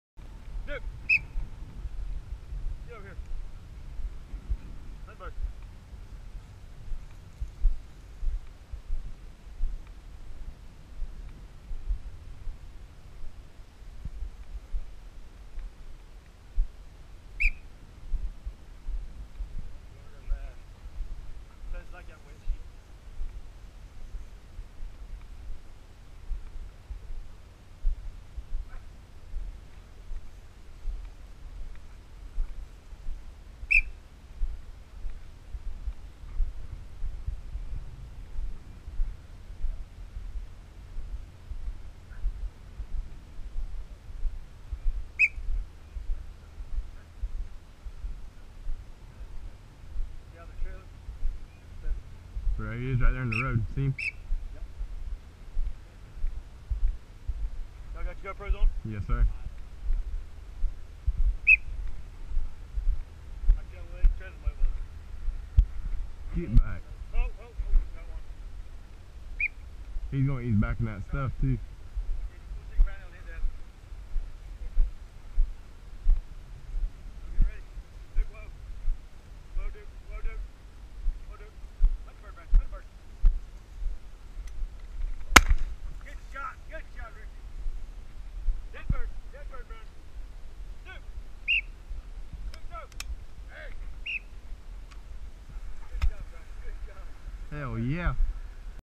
Afternoon Pheasant Hunt at Pine Ridge Plantation in Okeechobee, FL
As things settled down for the day we decided to head out to work the dogs and see if we could jump a few pheasants.
Pine Ridge Plantation OKeechobee, FL